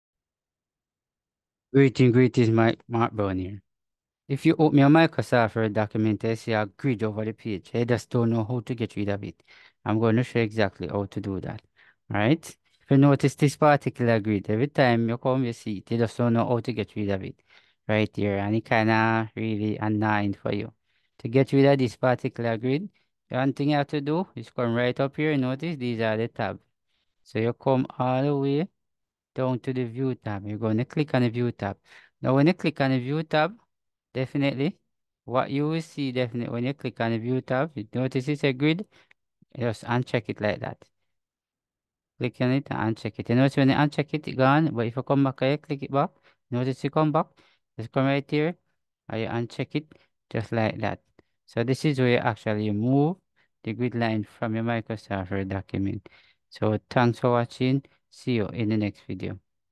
How to remove Grids In Microsoft Word
This video is to show you'll how to remove grids every time when you open your Microsoft office. I know its irritating when you can't find a way to solve this problem. Well here a good news. It's easy to remove it and get back to normal.

The most likely reason is that the drawing grid has been turned on. Follow these steps to turn it off: Make sure the View tab of the ribbon is displayed. Clear the Gridlines check box in the Show/Hide group.